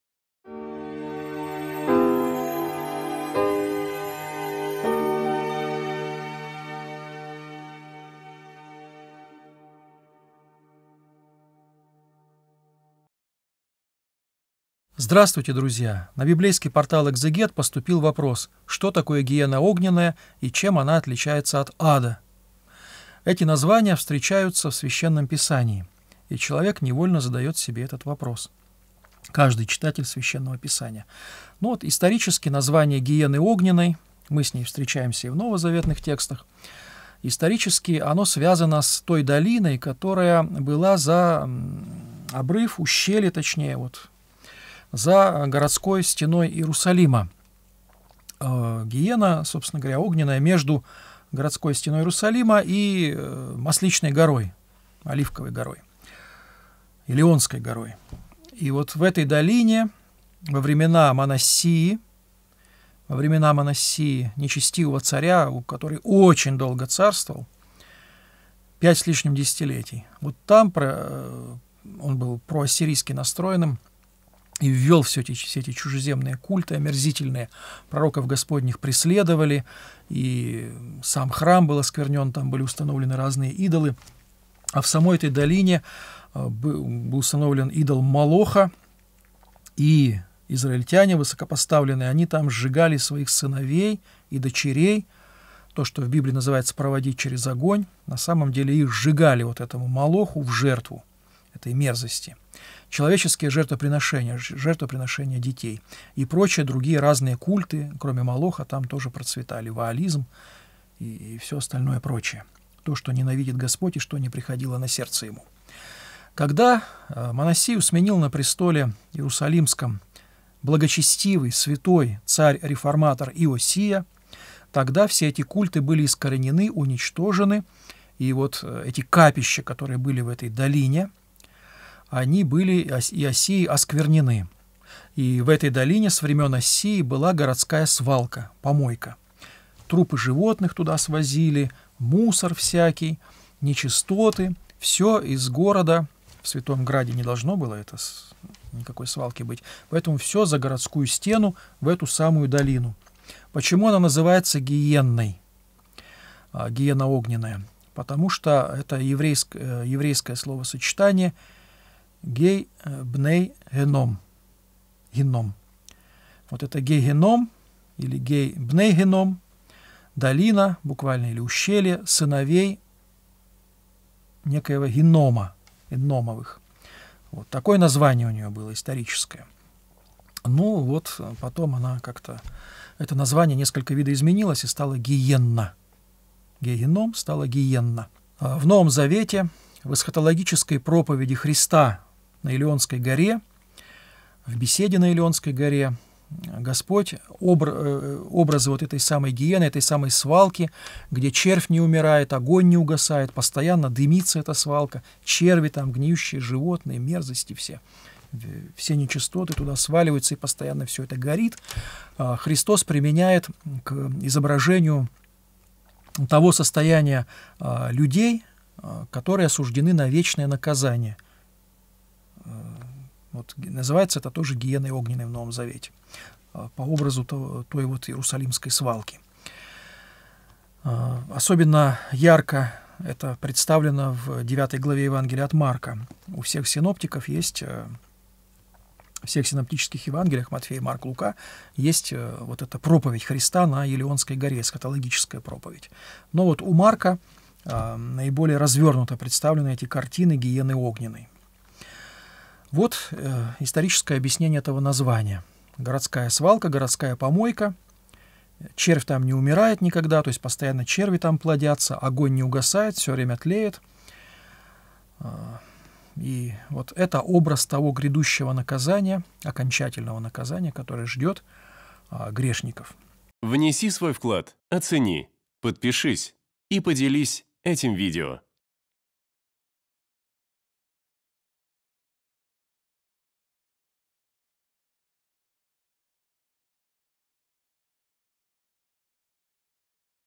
ГЕЕННА ОГНЕННАЯ. Что это такое ? Игумен Арсений (Соколов)
Команда Экзегет.

В сегодняшнем видео с игуменом Арсением (Соколовым) рассуждаем на тему:
" Геенна огненная"

🔸   Долина за городской стеной Иерусалима
🔸   Жертвоприношения при царе Манассии
🔸   Геенна огненная – свалка мерзости и нечистот Иерусалима
🔸   Сравнение невыносимости бытия с геенной огненной